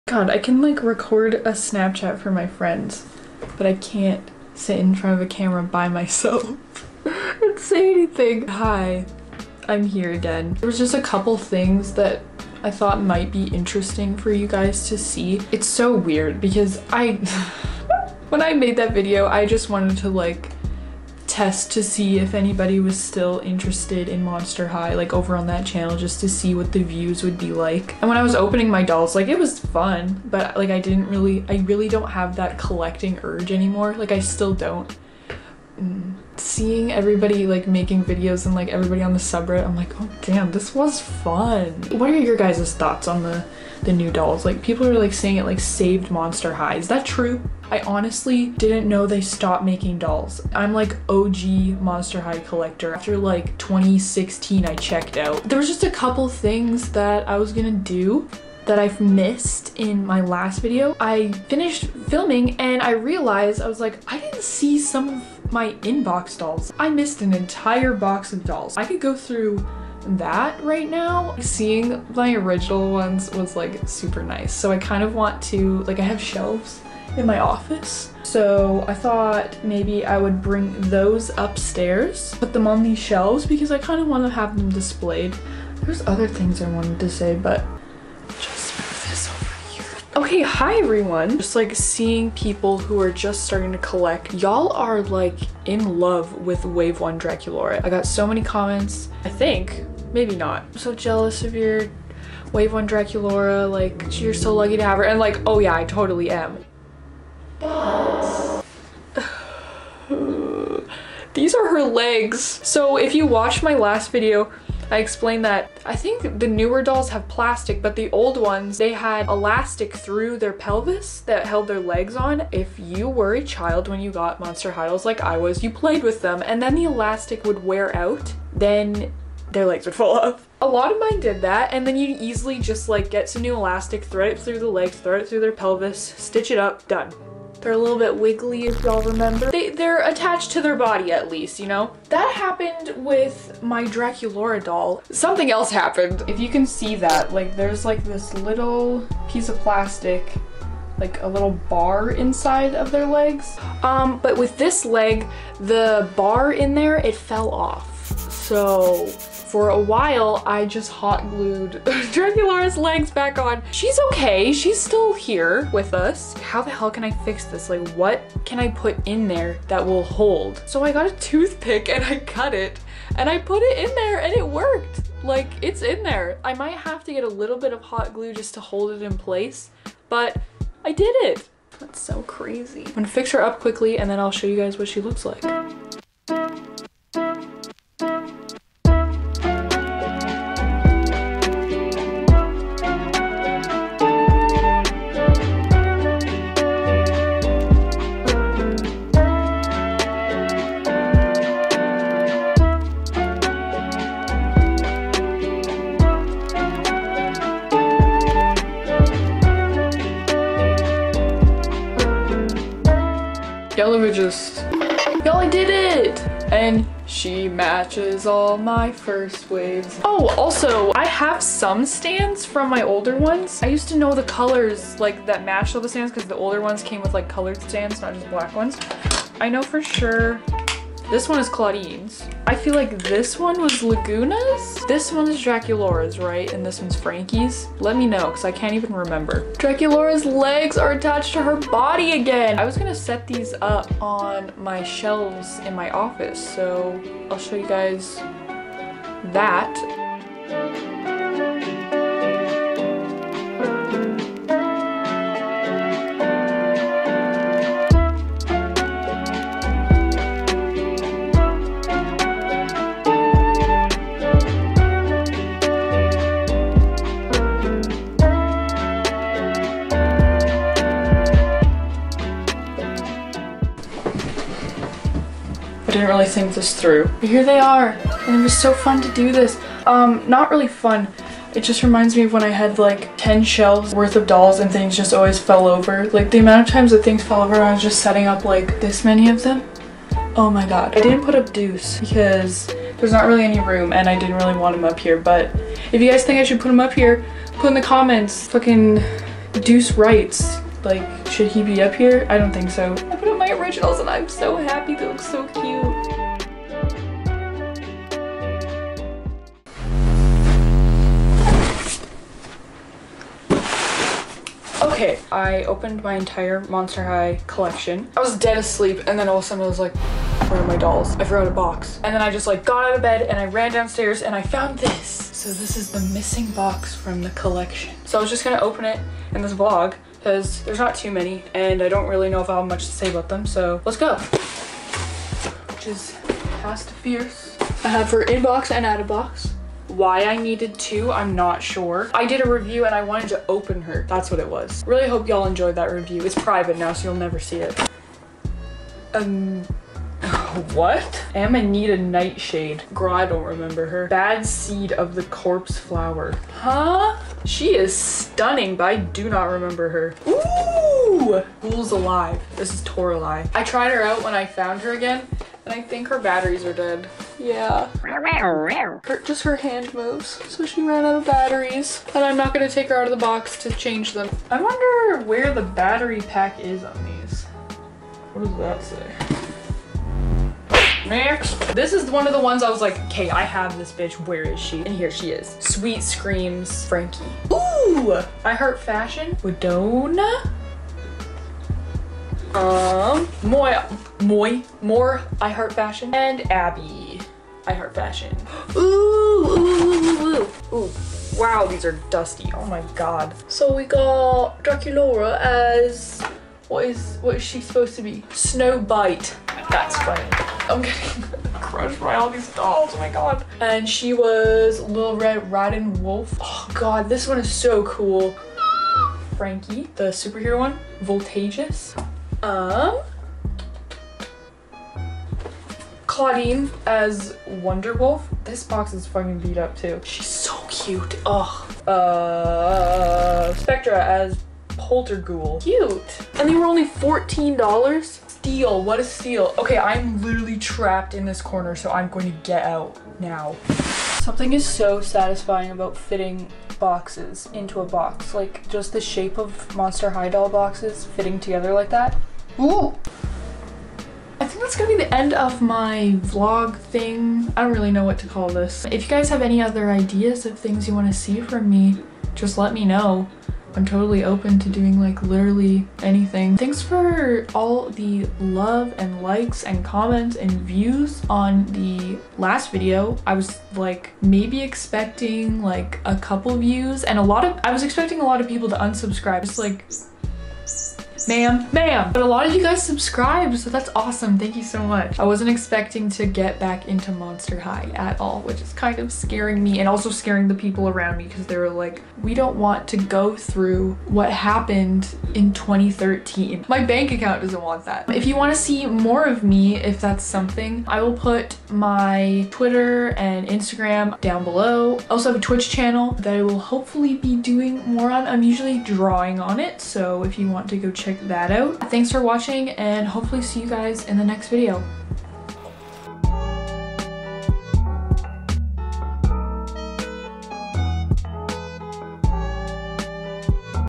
The Missing Box from my Monster High Collection (VLOG)
Thought I had too many dolls before? Think again. Opening the missing box from my collection, fixing some old dolls and just vibin!

Tag yourself, I'm the camera that won't focus.